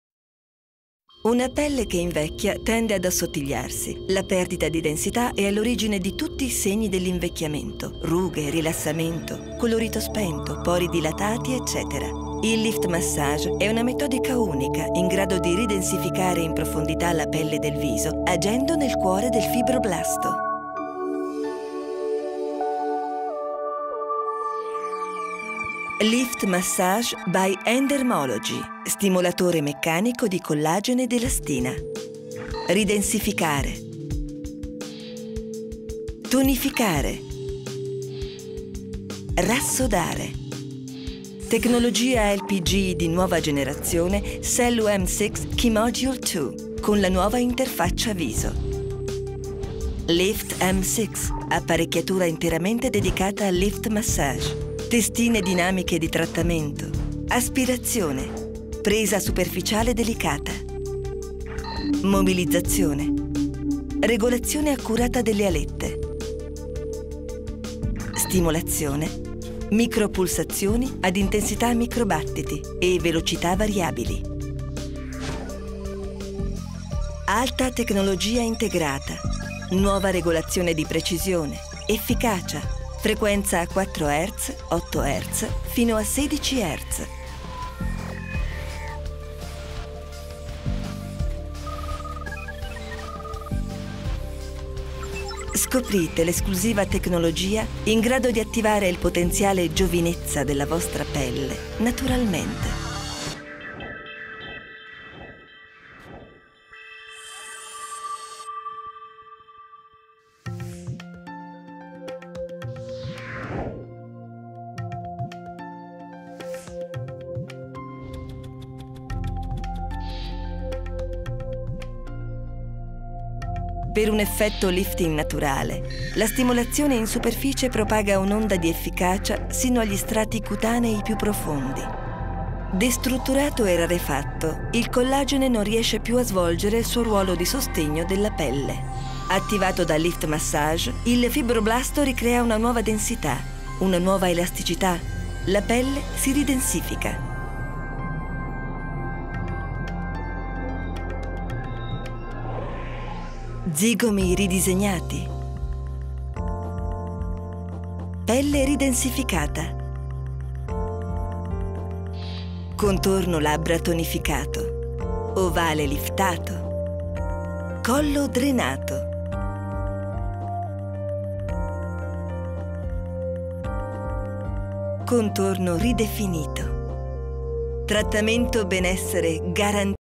Figurella Barletta - Lift Massage
ENDERMOLIFT
Questa esclusiva metodica anti-aging consente di ridensificare la pelle in profondità, cancellando i segni del tempo. Le testine Lift LPG grazie all'azione delle alette stimolano e risvegliano in profondità le cellule che sono responsabili della produzione naturale di collagene ed elastina.